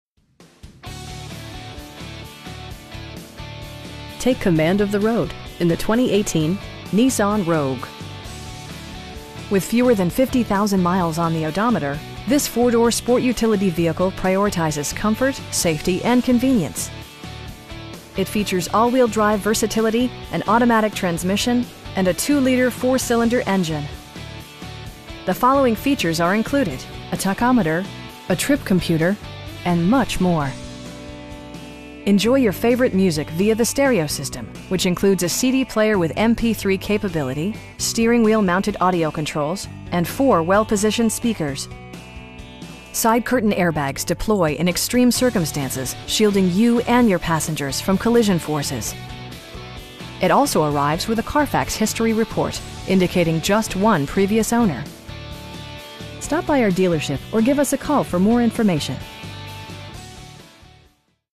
2018 Nissan Rogue Sport 2018.5 Sawd in Alcoa, TN 37701
Airport Honda
3069 Alcoa Highway

Take command of the road in the 2018 Nissan Rogue. With fewer than 50,000 miles on the odometer, this 4 door sport utility vehicle prioritizes comfort, safety and convenience. It features all-wheel drive versatility, an automatic transmission, and a 2 liter 4 cylinder engine. The following features are included: a tachometer, a trip computer, air conditioning, power door mirrors, power windows, an overhead console, and much more. Side curtain airbags deploy in extreme circumstances, shielding you and your passengers from collision forces. It also arrives with a Carfax history report, indicating just one previous owner. We have the vehicle you've been searching for at a price you can afford. Stop by our dealership or give us a call for more information.